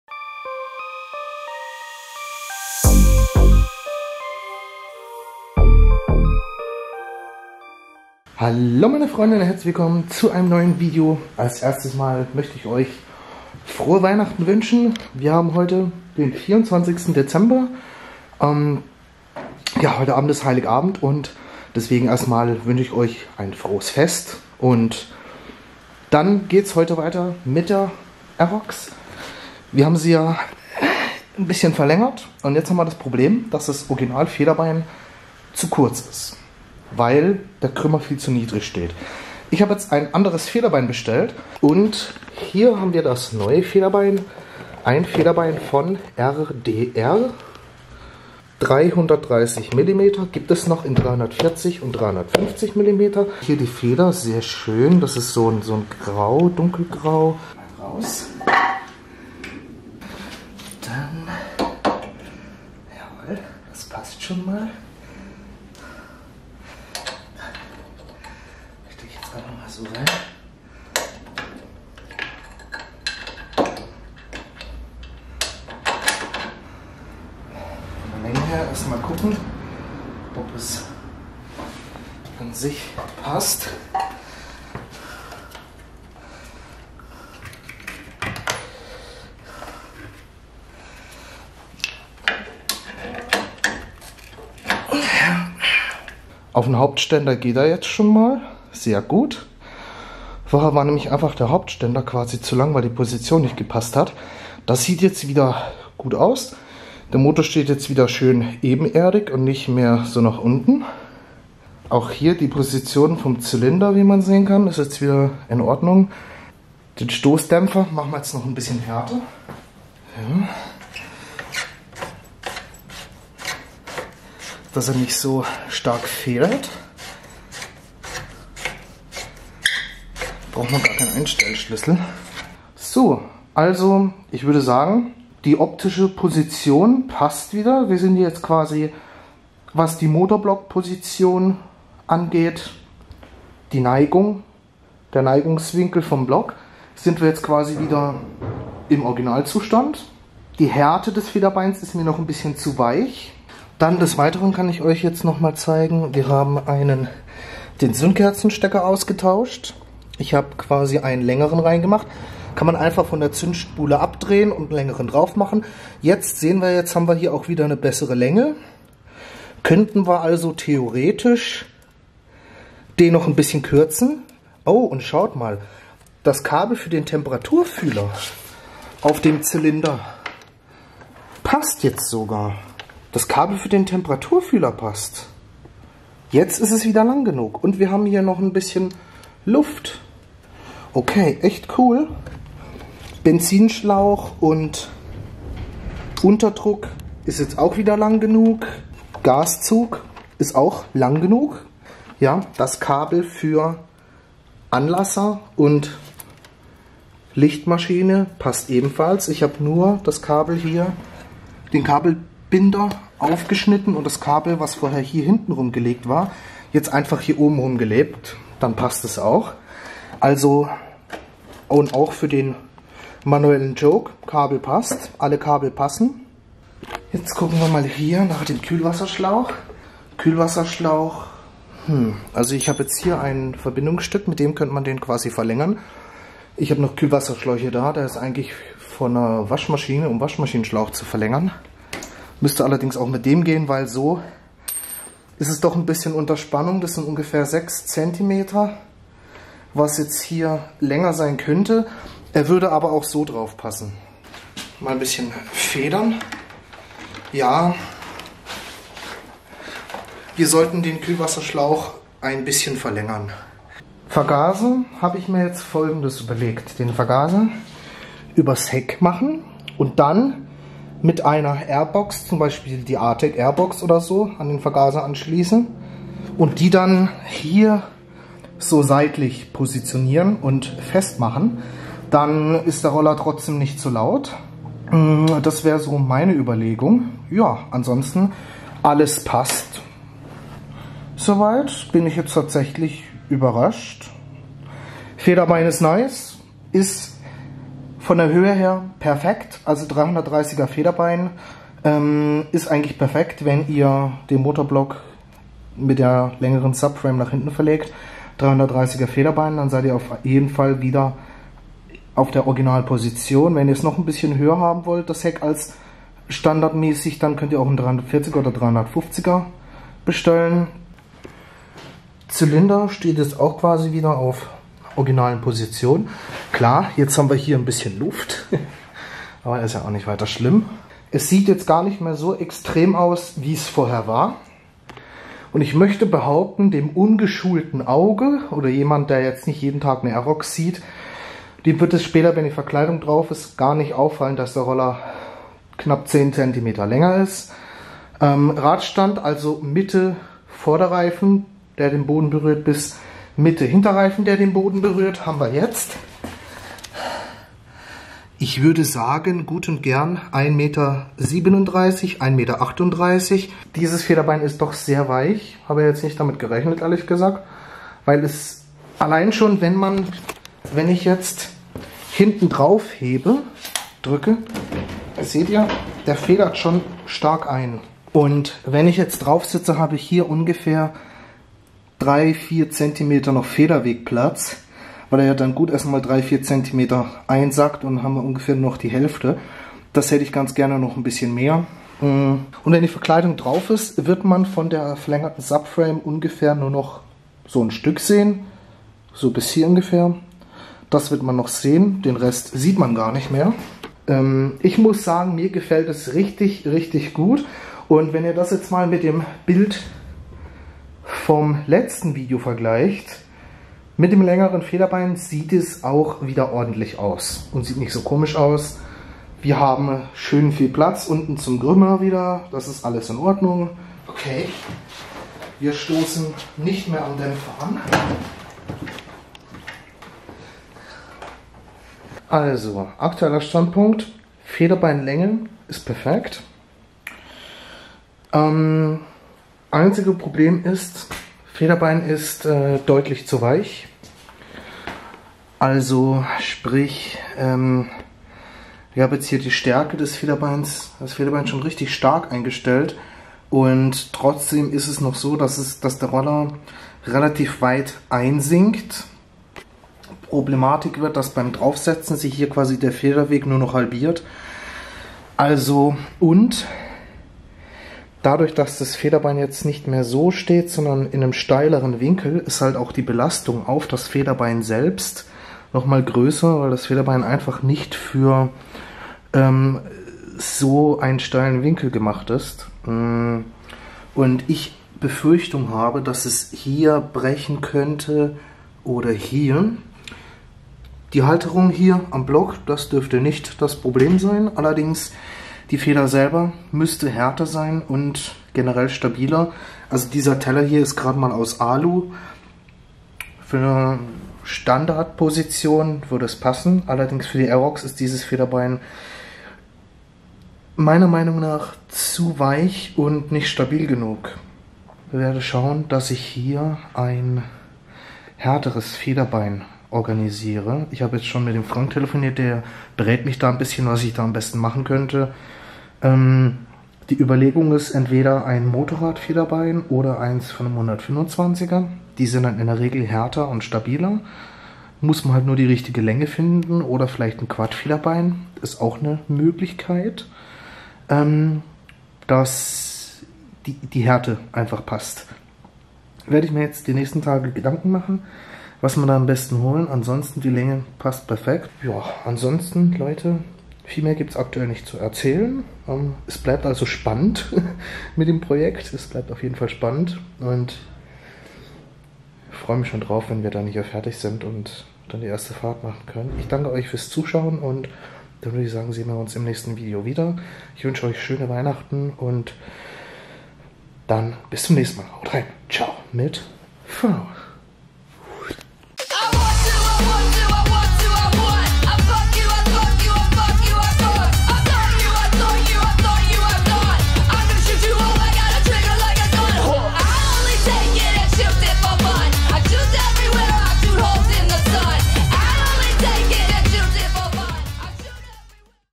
RDR Federbein 330mm länge passt aber viel zu weich| Aerox Verlängern
Das Projekt Aerox verlängern bleibt spannend :)